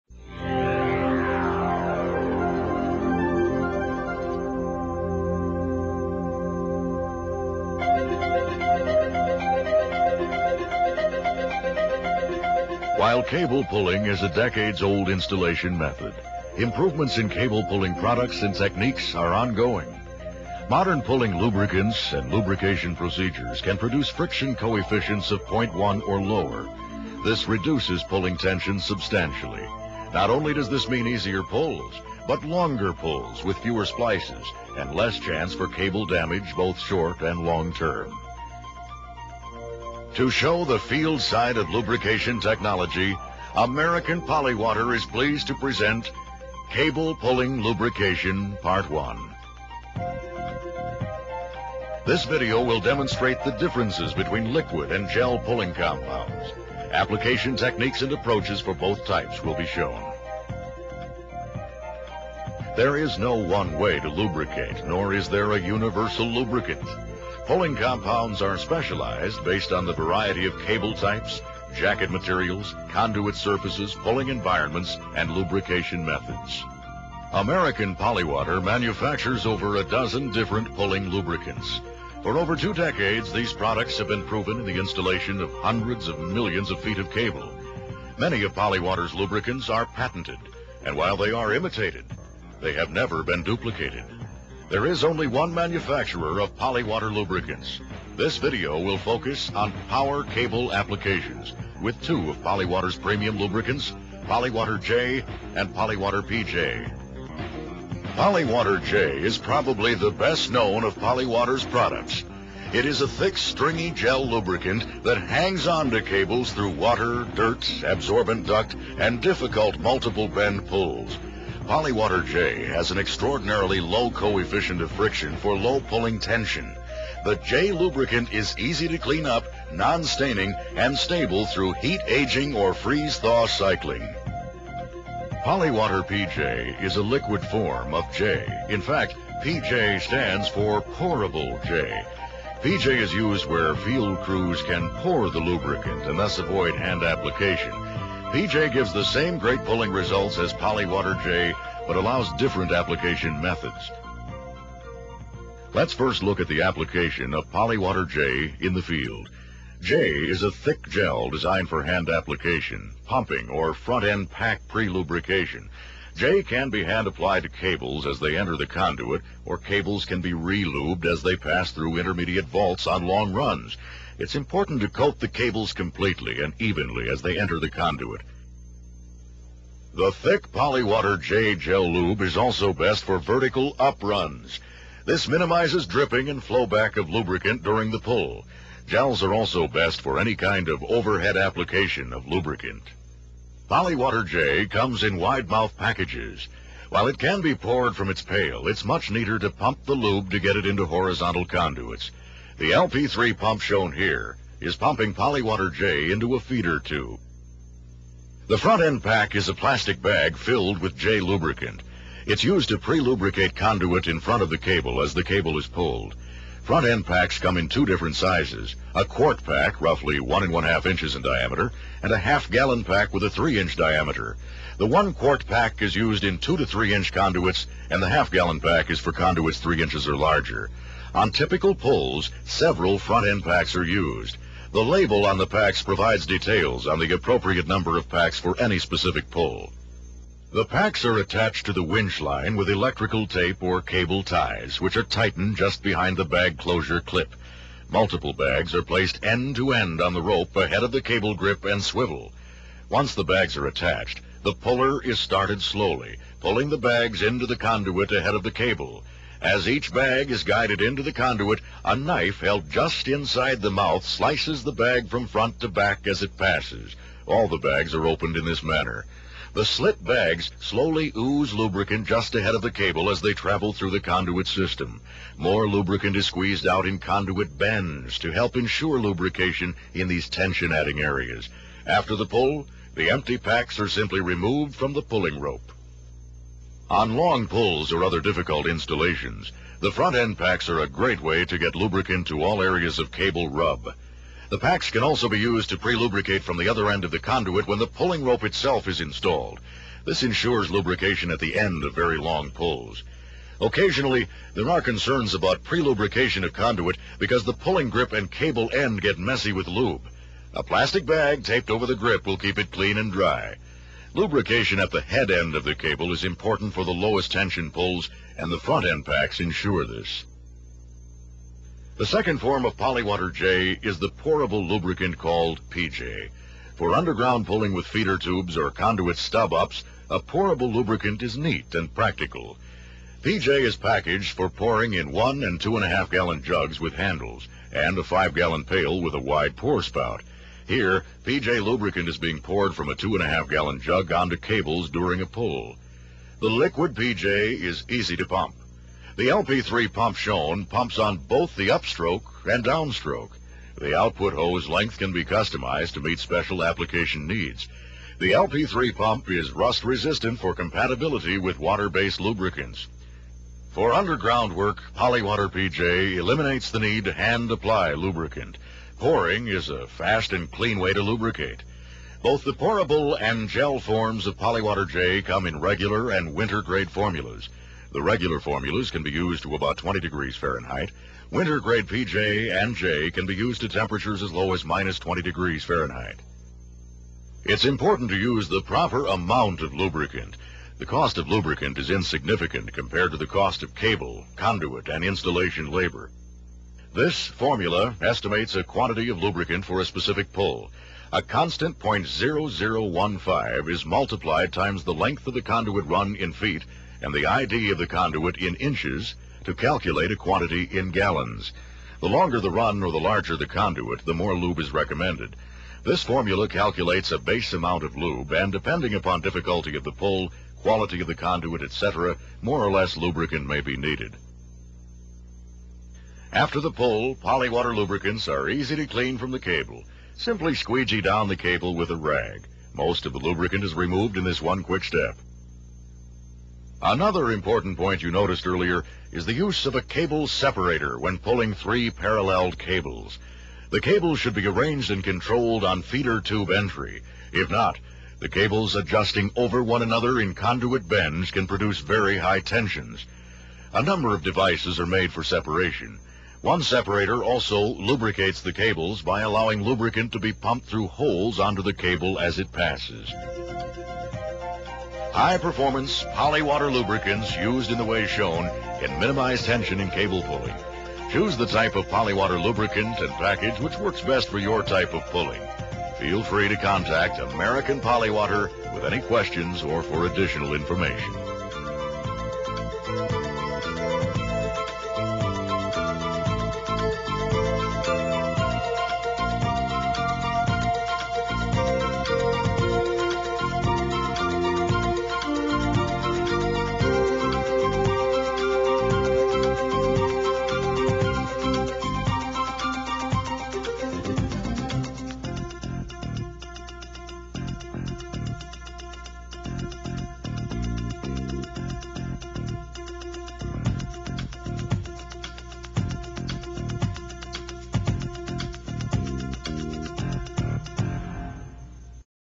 Cable Pulling Lubrication
training video demonstrates cable pulling lubrication procedures during high voltage electrical installations.   The differences between gel and liquid pulling compounds are featured.   Techniques for applying lubricant by pump, prelubricating with Front End Packs™, cold weather pulling, determining how much lubricant to use, and cleaning up are presented through actual field installation footage.